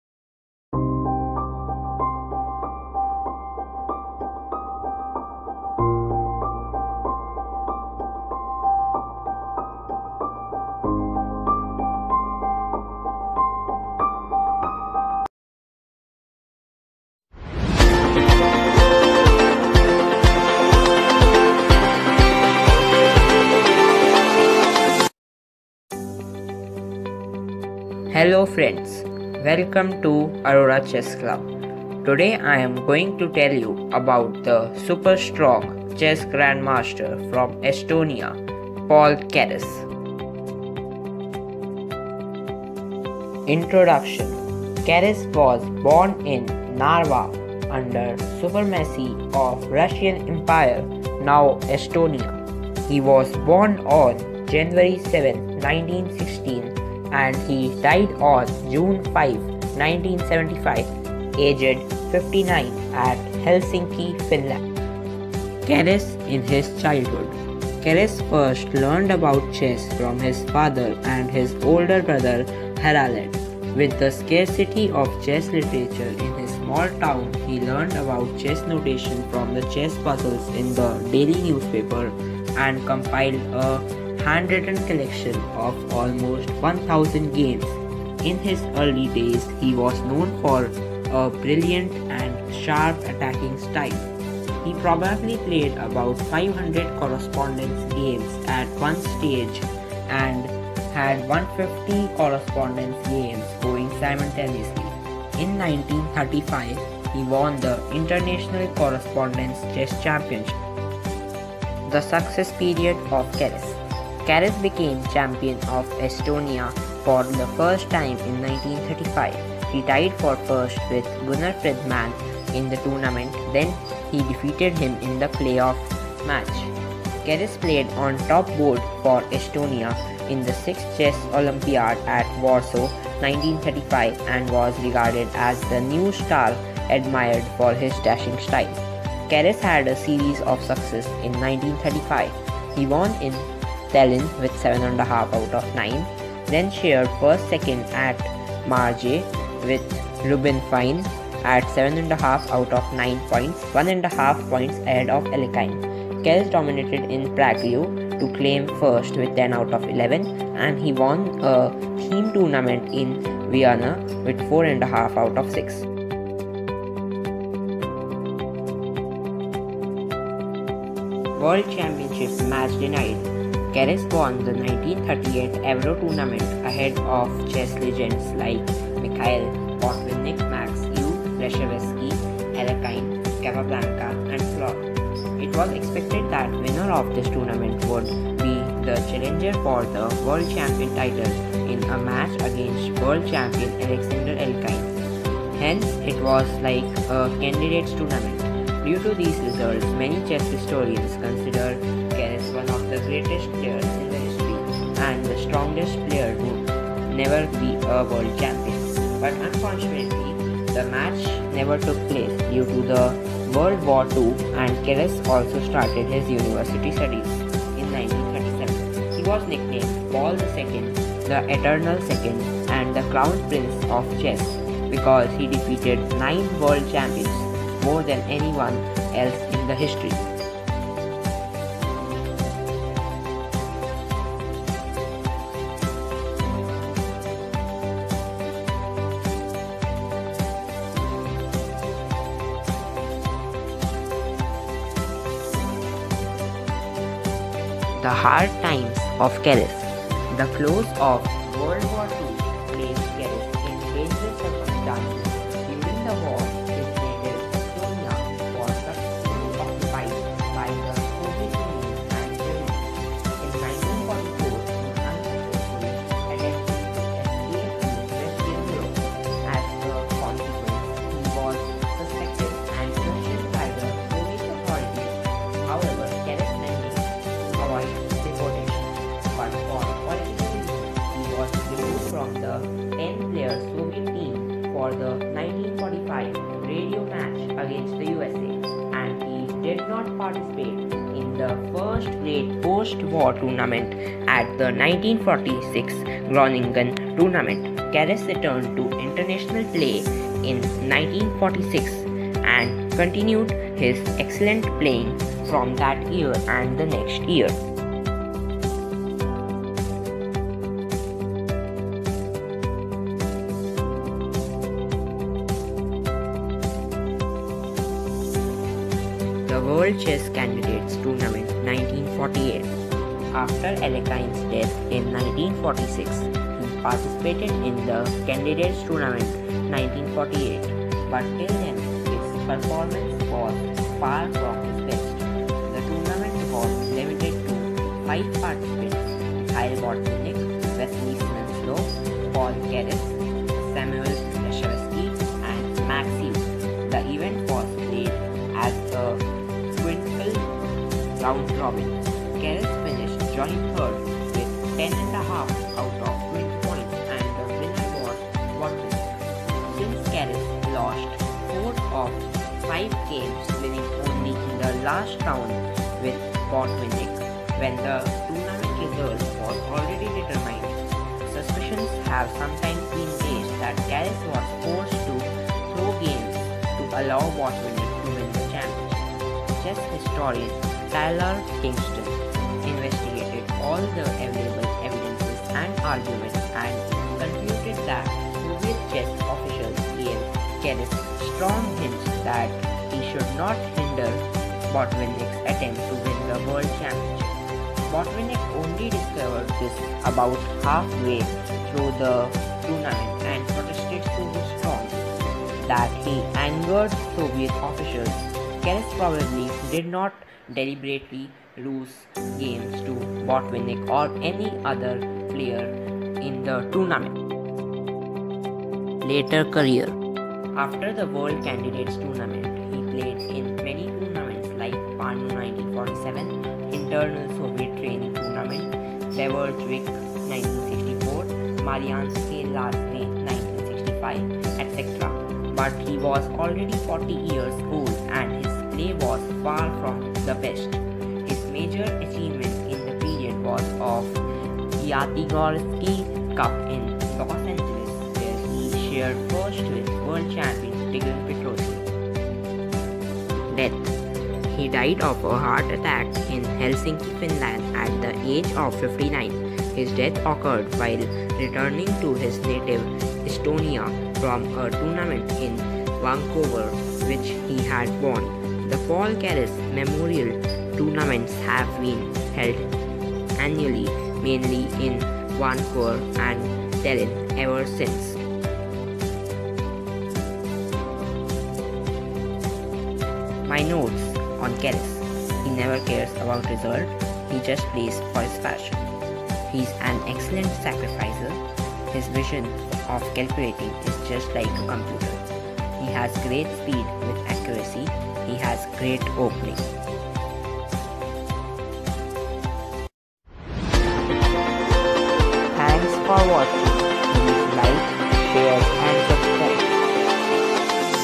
Biography of Paul Keres || Honi Arora
Hello Friends , welcome to ACC [Arora Chess Club] ! My Name is Honi Arora. Today I am going to tell you about a great Estonian chess Grandmaster Paul Keres .I will be shortly uploading many more videos on Biography of players.